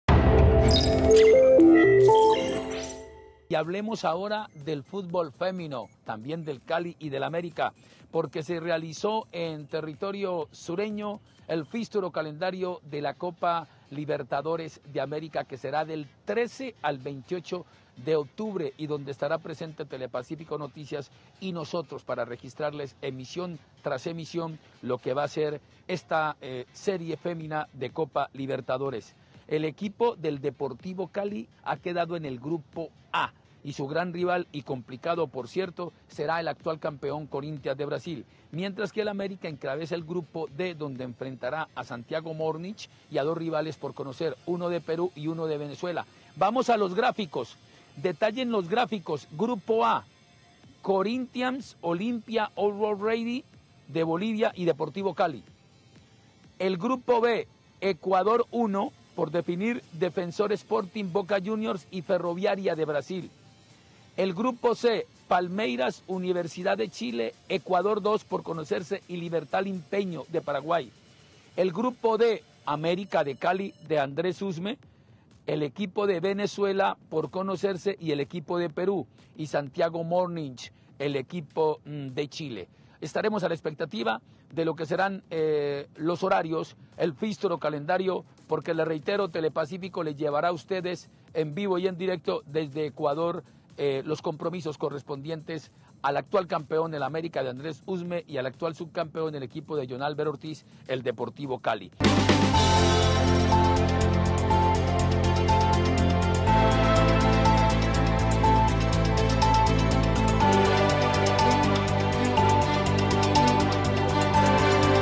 Copa Libertadores Femenina 2022 ya tiene calendario
Deportes | Se dio a conocer la lista de grupos para la Copa Libertadores Femenina 2022, que tendrá lugar en Ecuador, del 13 al 28 de octubre.